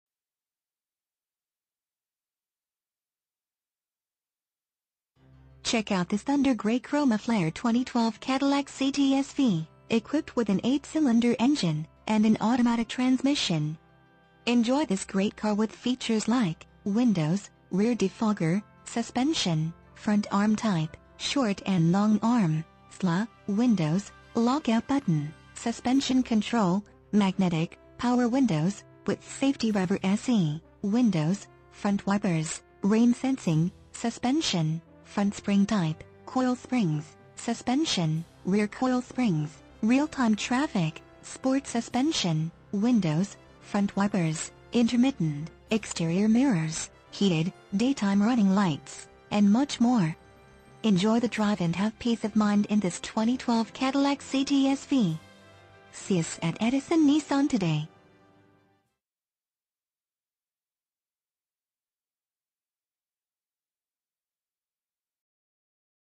2012 Cadillac CTS-V Edison Nissan Edison, NJ 08817
Cadillac CTS-V Imagine driving this Thunder Gray Chromaflair Cadillac Cts-V 4 Dr Sedan, equipped with an 8 Cylinder engine and an automatic transmission.
MILES: 18,279
VIN:1G6DV5EP0C0117722

Edison Nissan
401 Route 1 South
Edison, New Jersey 08817